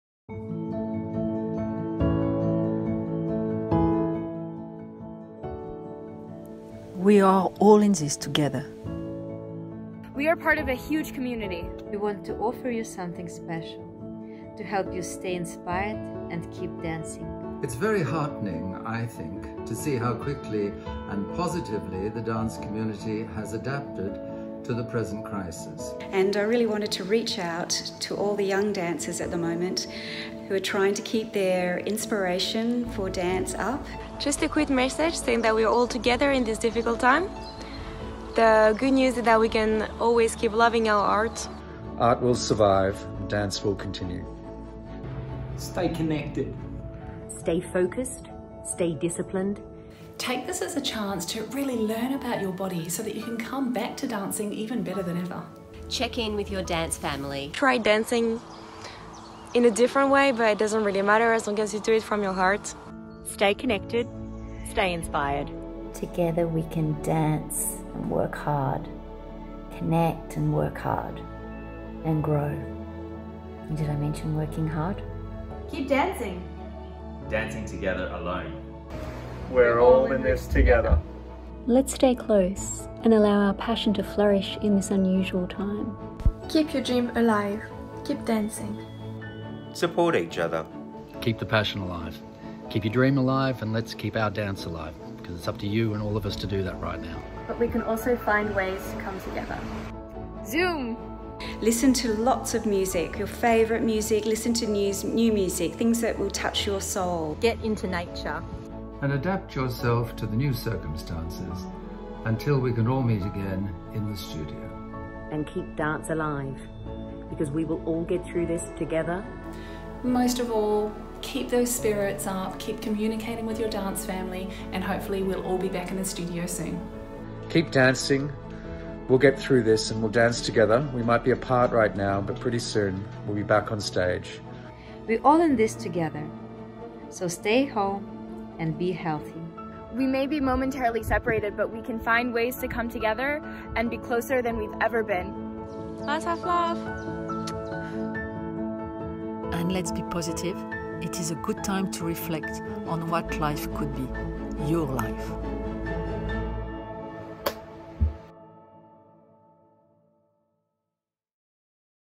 A message of support from our dance family
We asked our beautiful dance family from around the world to send messages of support and encouragement during this challenging period and made a video for you with their inspiring messages. 💗

"Whilst we may be momentarily separated, we can come together and be closer than we've ever been."

With love
~ MDM Team x

our thanks to: Sylvie Guillem, Evgenia Obraztsova, Stanton Welch, Juliet Doherty, Ines McIntosh, Chloe Reveillon, John Byrne, Lisa Bolte, Vicki Attard, Bradley Chatfield, Olivia Bell, Kirsty Martin, Lisa Howell, Simone Sault, Leanne Stojmenov, Lana Jones, Daniel Gaudiello, Marc Cassidy, Lucy Doherty, Rob McLean, Josef Brown.